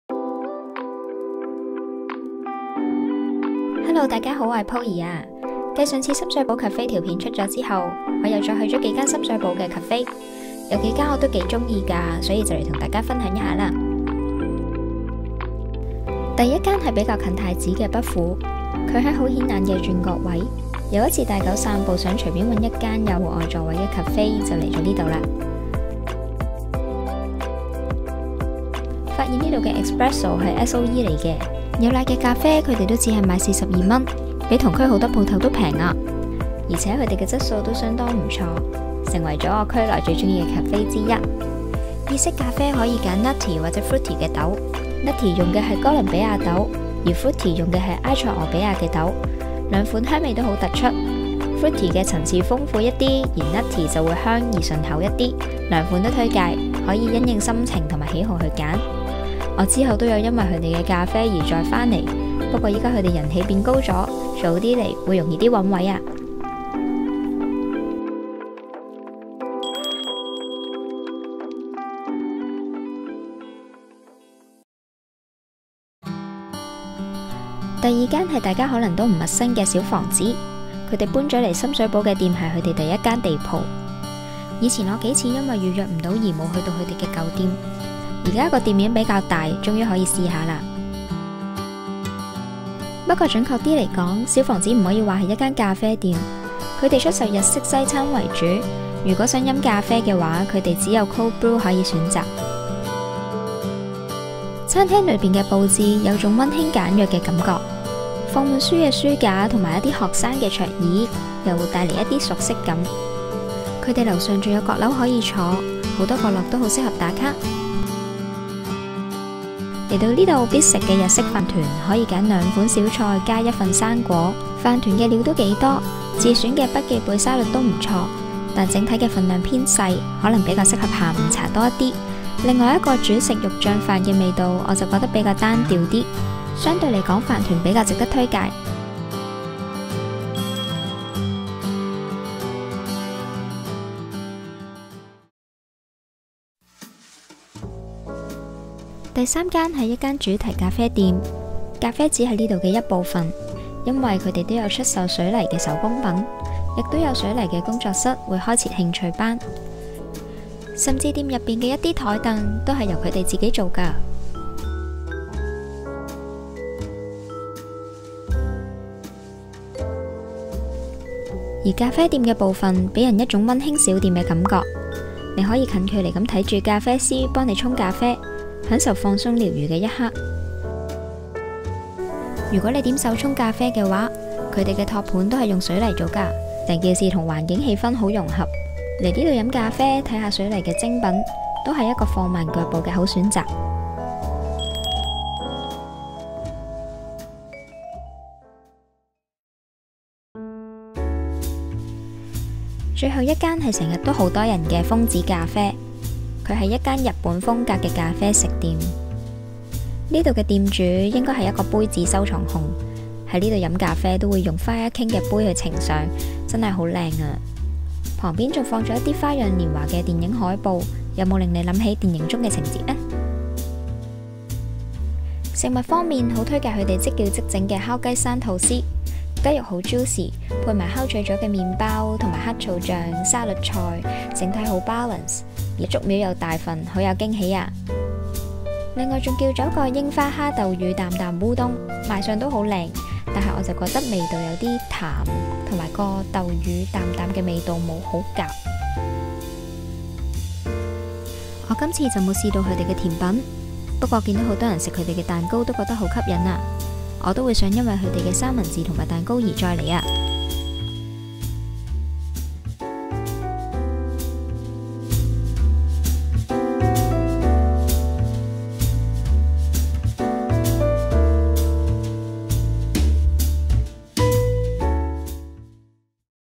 香港cafe推介EP.2☕️｜再發掘4間特色深水埗咖啡店，分享近期愛店｜日系打卡、水泥精品、SOE咖啡店｜Hong Kong Sham Shui Po Cafe 2
第二集香港cafe推介，也是深水埗區的，店的資料放在下面🥰

不苦 Buff
深水埗楓樹街5A號

小房子
深水埗大南街196號地舖

FREEZING IN.
深水埗基隆街98C號地舖

楓子珈琲 Fonji Cafe
深水埗楓樹街5號